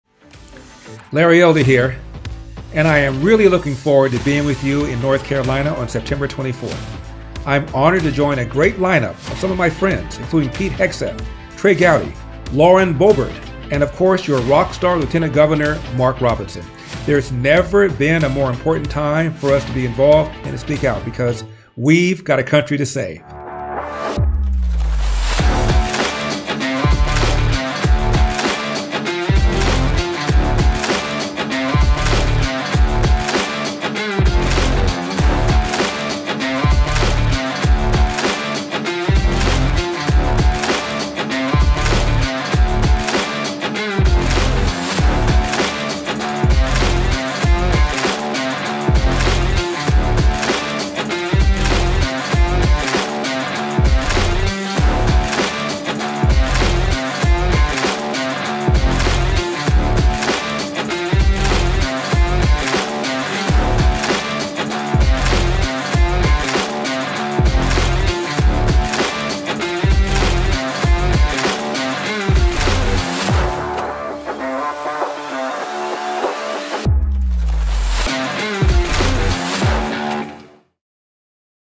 Larry Elder Coming to Salt & Light
The Sage from South Central Los Angeles is coming to Charlotte this weekend because together "we've got a country to save!" Know as the "firebrand according to “Daily Variety,” Larry Elder is an American libertarian talk radio host, author, attorney, and documentary filmmaker who hosts The Larry Elder Show is coming to the Salt & Light Conference.

His ironic radio show began as a local program on Los Angeles radio station KABC in 1993 and is now nationally syndicated. Larry shines the bright light of reasoned analysis on many of the myths and hypocrisies apparent in our system of government, our society, and the media itself. He slays dragons and topples sacred cows using facts, common sense, and a ready wit. He was most recently a Republican gubernatorial candidate in California’s recall election and just yesterday at the Iowa Faith & Freedom event he hinted that he has his sights on a national campaign in 2024.

The Salt & Light Conference kicks off Friday night at 6pm. Do you have your ticket?